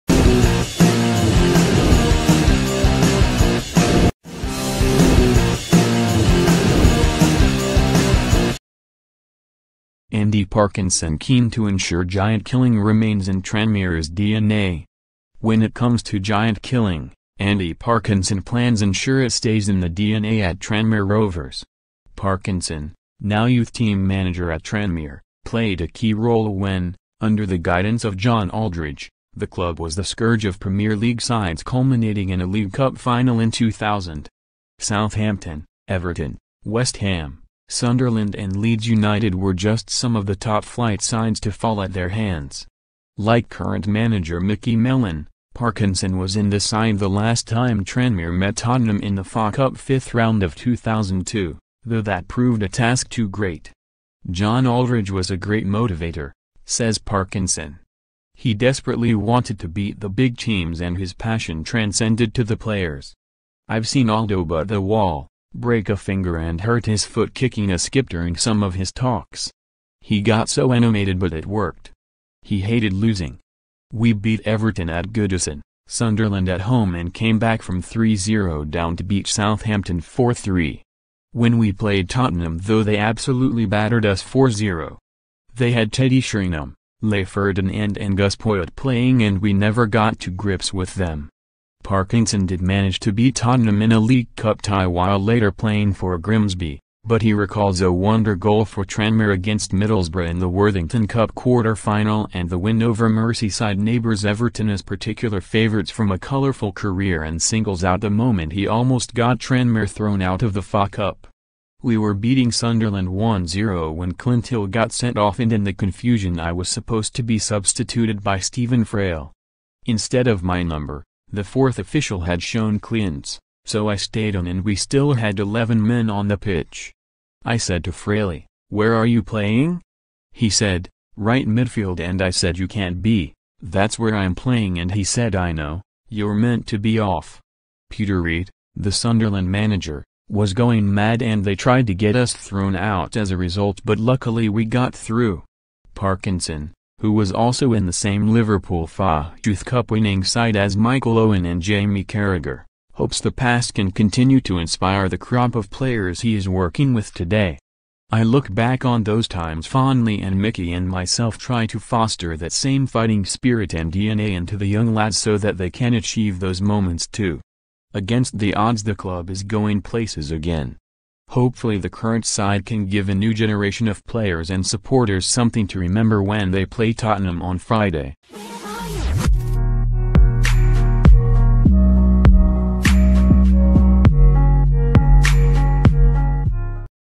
Andy Parkinson keen to ensure giant k illing remains in Tranmere's DNA
When it comes to giant-killing, Andy Parkinson plans ensure it stays in the DNA at Tranmere Rovers.

Parkinson, now youth team manager at Tranmere, played a key role when, under the guidance of John Aldridge, the club was the scourge of Premier League sides culminating in a League Cup final in 2000.

Southampton, Everton, West Ham, Sunderland and Leeds United were just some of the top flight sides to fall at their hands. Like current manager Micky Mellon, Parkinson was in the side the last time Tranmere met Tottenham in the FA Cup fifth round of 2002, though that proved a task too great.

'John Aldridge was a great motivator,' says Parkinson. 'He desperately wanted to beat the big teams and his passion transcended to the players.

'I've seen Aldo butt the wall, break a finger and hurt his foot kicking a skip during some of his talks. He got so animated but it worked. He hated losing.

'We beat Everton at Goodison, Sunderland at home and came back from 3-0 down to beat Southampton 4-3. When we played Tottenham though they absolutely battered us 4-0. They had Teddy Sheringham, Les Ferdinand and Gus Poyet playing and we never got to grips with them.'

Parkinson did manage to beat Tottenham in a League Cup tie while later playing for Grimsby, but he recalls a wonder goal for Tranmere against Middlesbrough in the Worthington Cup quarter-final and the win over Merseyside neighbours Everton as particular favourites from a colourful career and singles out the moment he almost got Tranmere thrown out of the FA Cup.

'We were beating Sunderland 1-0 when Clint Hill got sent off and in the confusion I was supposed to be substituted by Stephen Frail. Instead of my number, the fourth official had shown Clint's, so I stayed on and we still had 11 men on the pitch. I said to Fraily: ''where are you playing?'' He said: ''right midfield'' and I said ''you can't be, that's where I'm playing'' and he said ''I know, you're meant to be off''.

Peter Reid, the Sunderland manager, was going mad and they tried to get us thrown out as a result but luckily we got through.' Parkinson, who was also in the same Liverpool FA Youth Cup winning side as Michael Owen and Jamie Carragher, hopes the past can continue to inspire the crop of players he is working with today.

'I look back on those times fondly and Micky and myself try to foster that same fighting spirit and DNA into the young lads so that they can achieve those moments too. Against the odds the club is going places again. Hopefully the current side can give a new generation of players and supporters something to remember when they play Tottenham on Friday.'